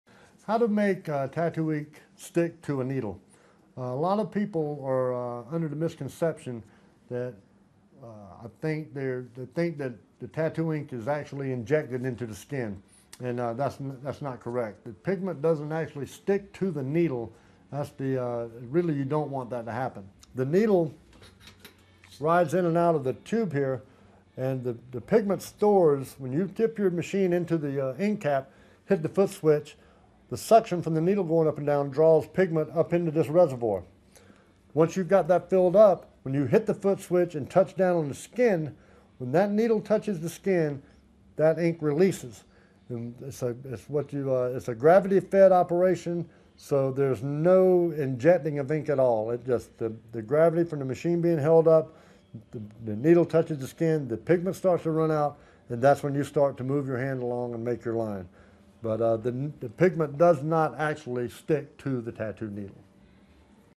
Make Ink Stick to a Tattoo Needle
Make Ink Stick to a Tattoo Needle. Part of the series: Tattooing. Ink does not stick to a tattoo needle, but it is drawn up into the tube by the movement of the needle before it runs down the side of the needle when it hits the skin. Find out how ink gets into the skin with helpful tips from an experienced tattoo artist in this free video on tattoo equipment.